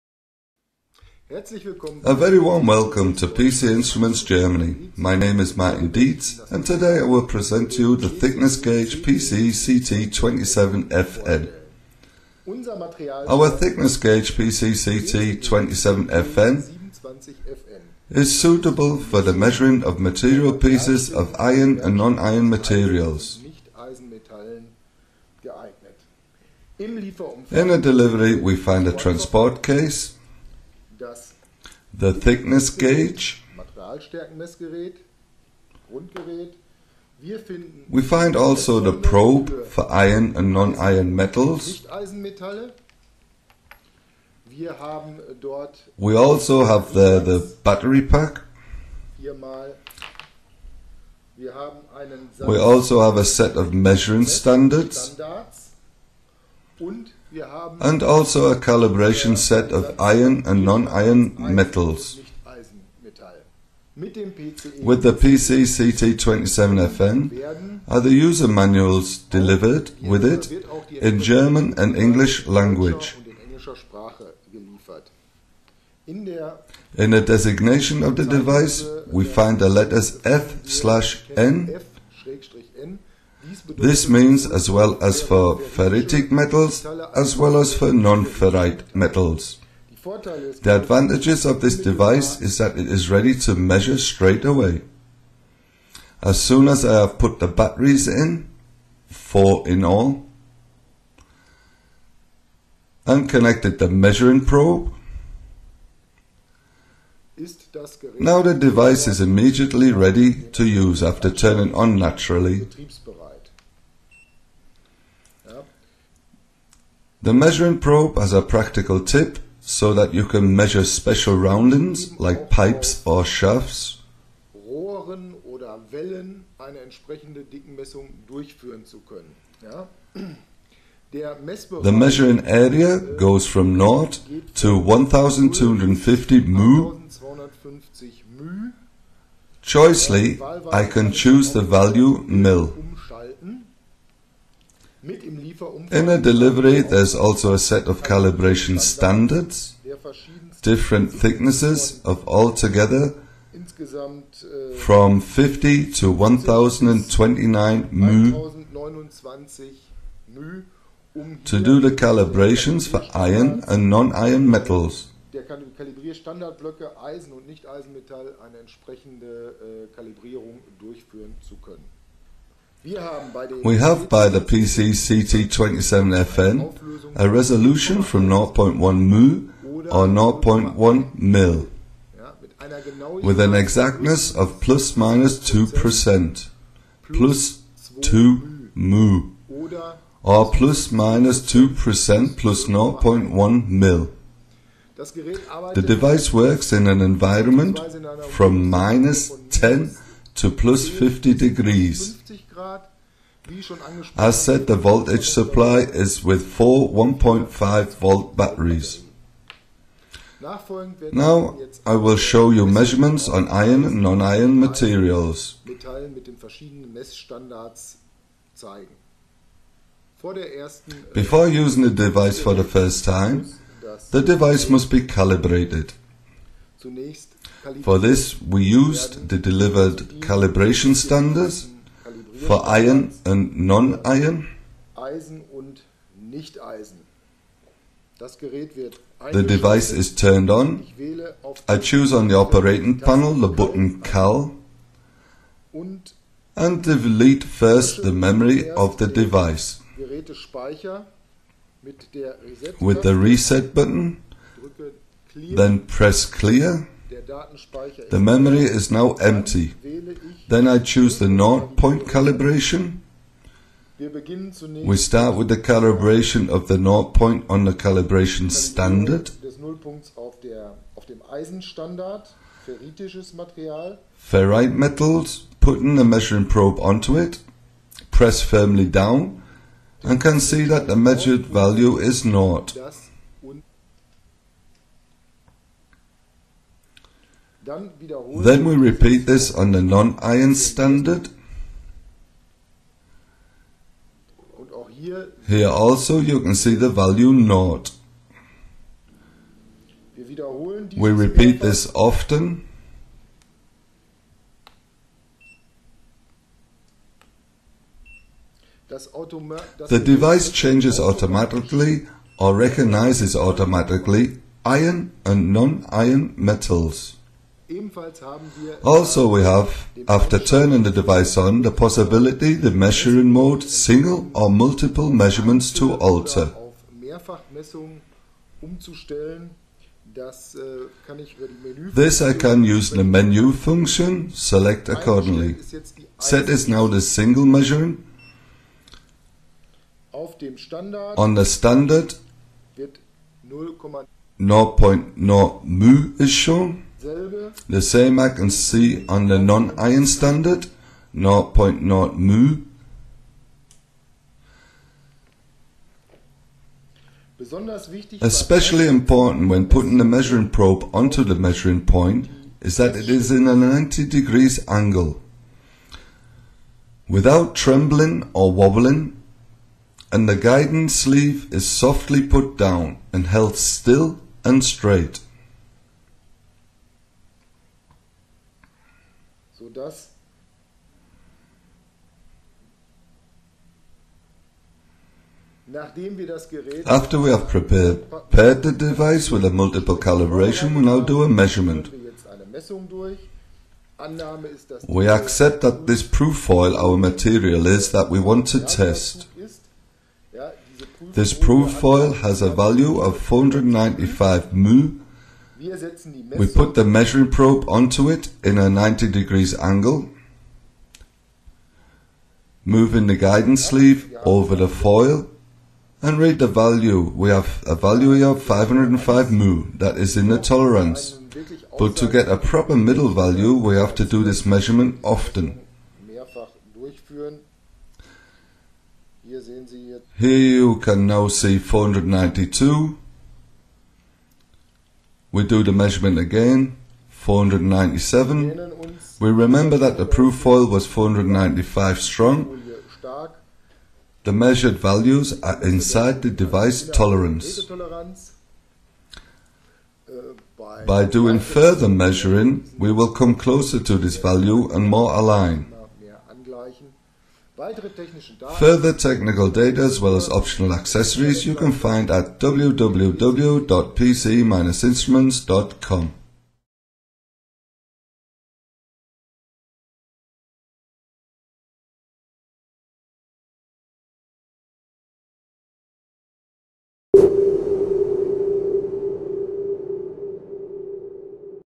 Material Thickness Meter PCE-CT 27FN determines the layer thickness.
In this video Martin Dietz introduces the NDT tester PCE CT 27FN.